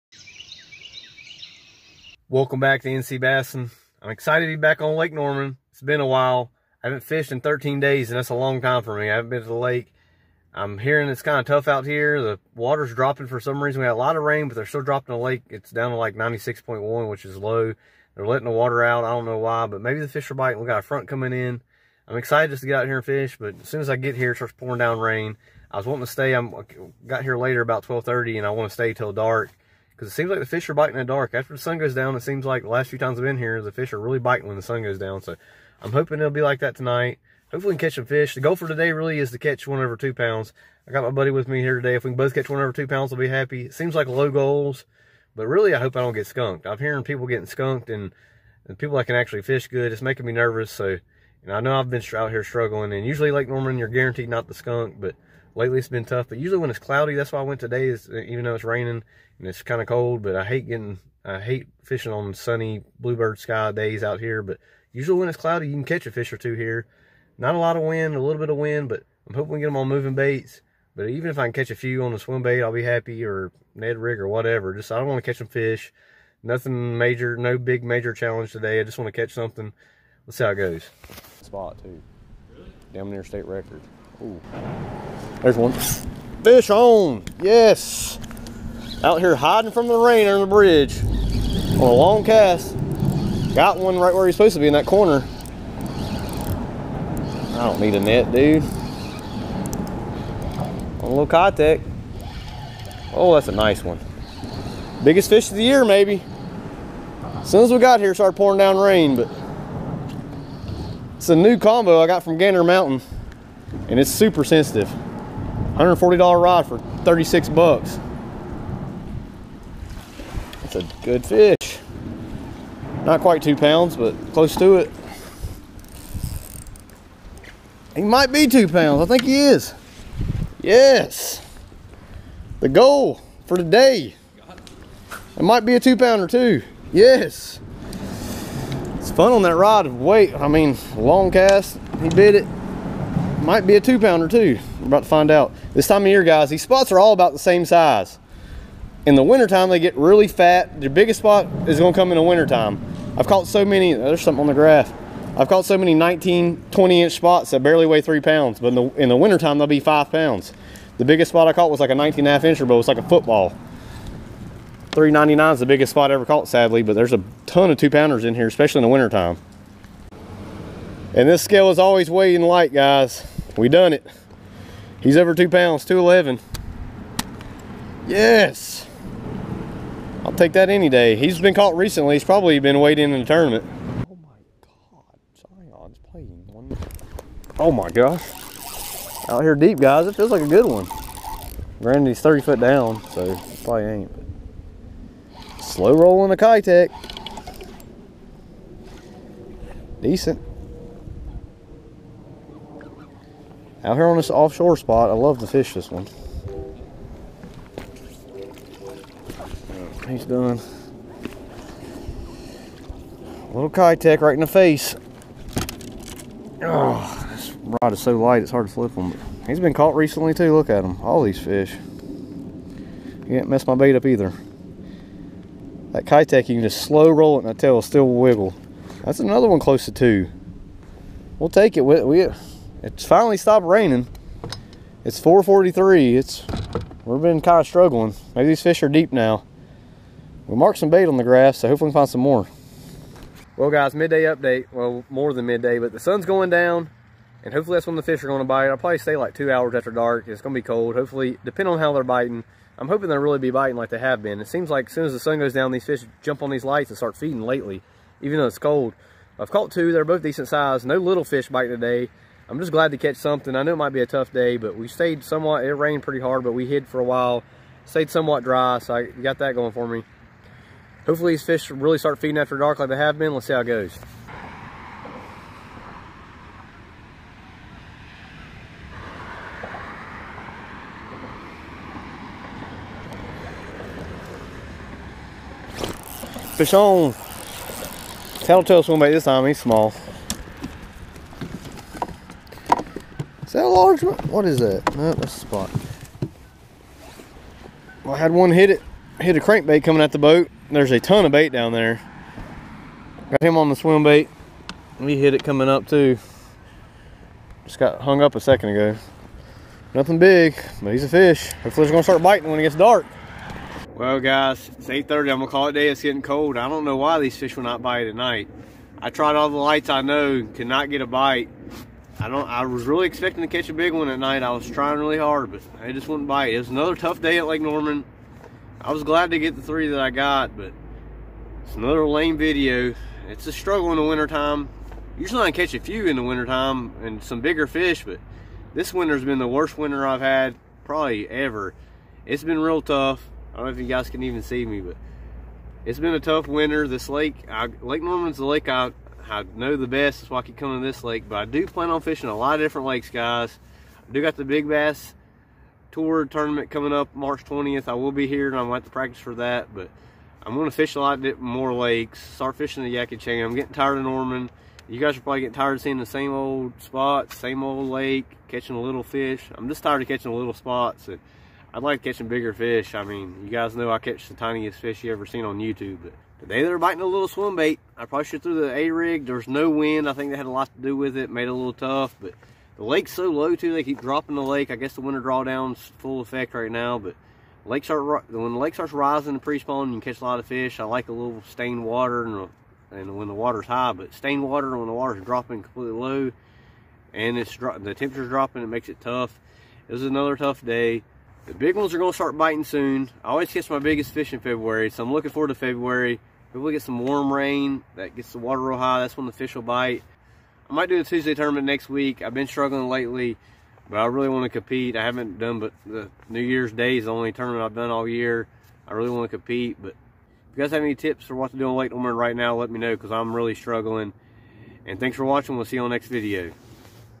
Winter spotted bass love small swimbaits
Water temp 50° main lake
Water visibility 3ft
Winds 2mph NW